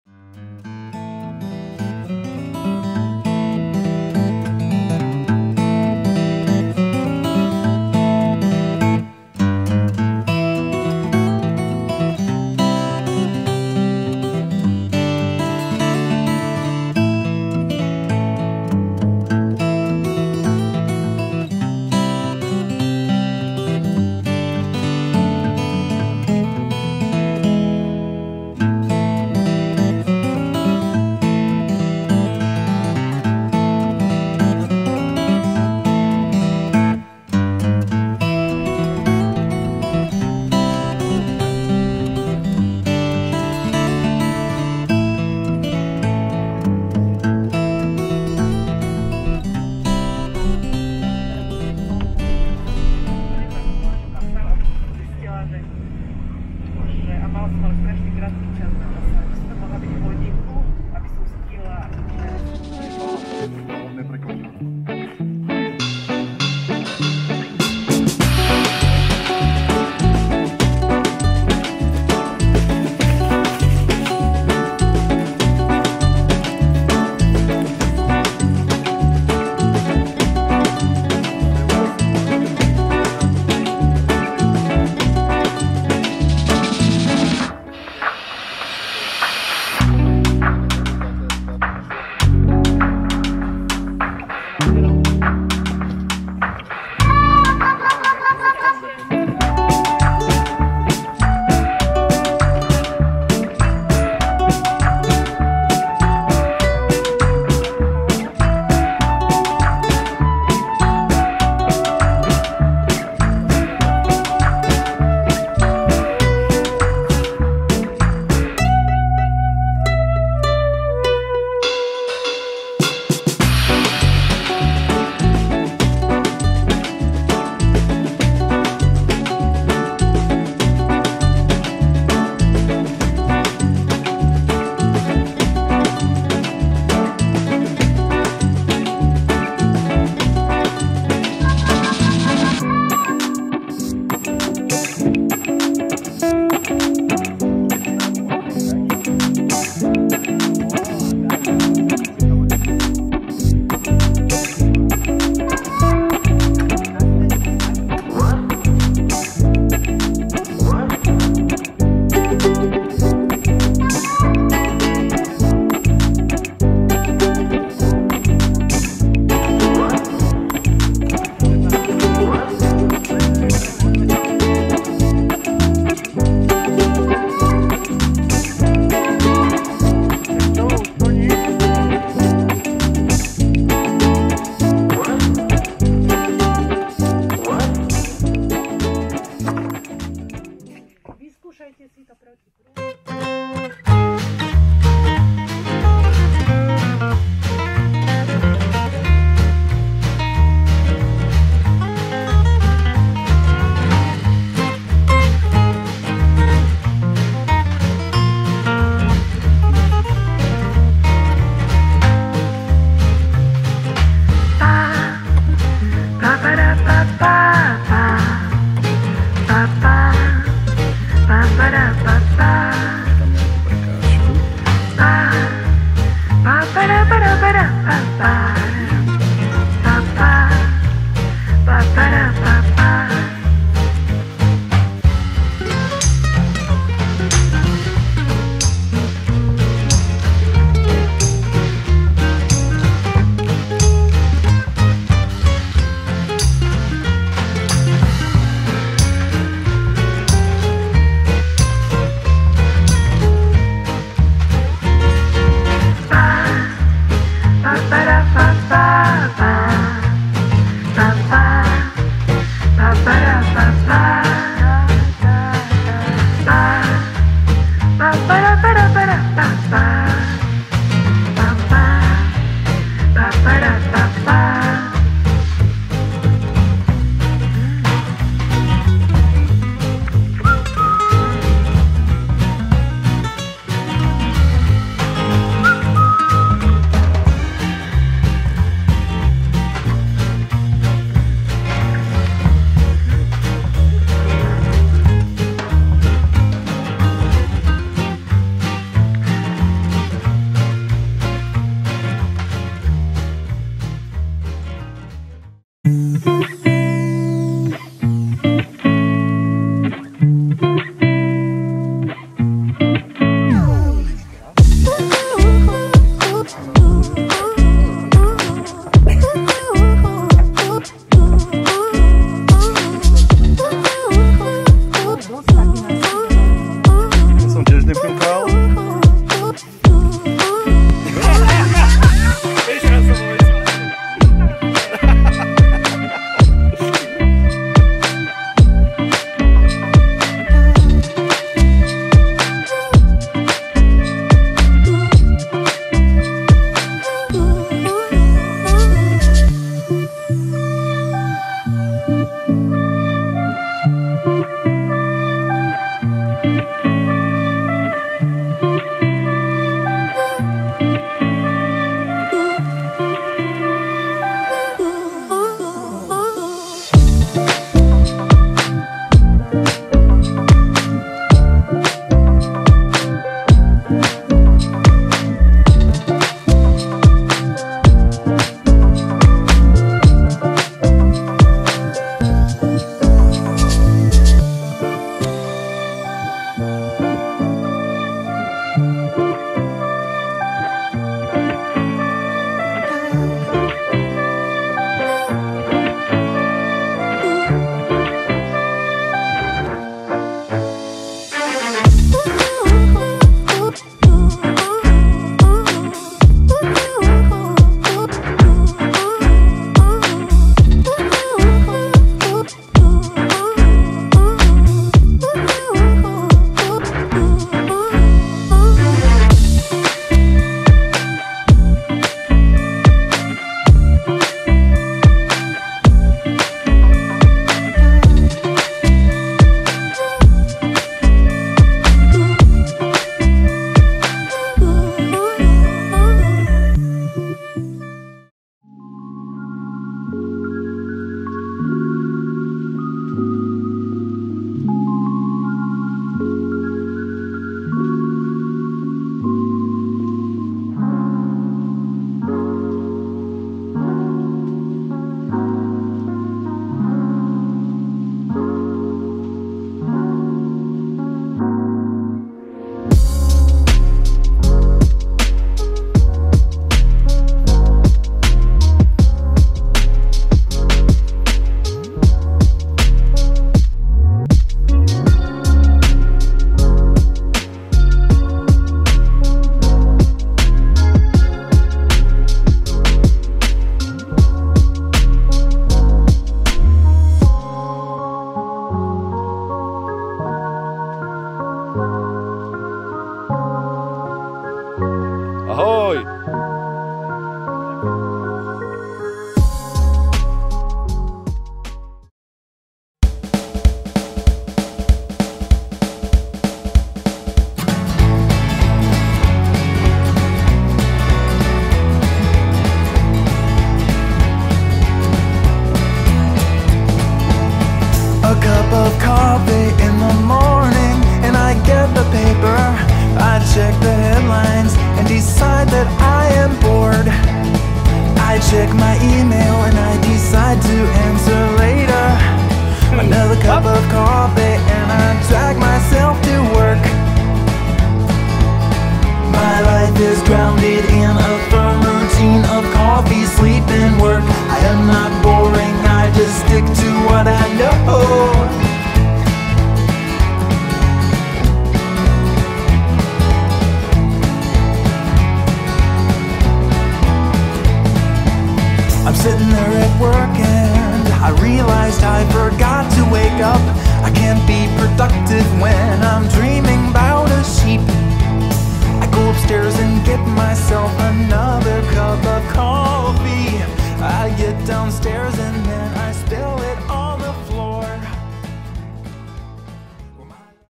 NotaBene splav 2021 HD 1080p
Každoročný splav s predajcami časopisu Nota Bena a s ivc kolegami s občianskeho združenia Proti prúdu. Vždy fantastická nálada, užastní ľudia a kamaráti.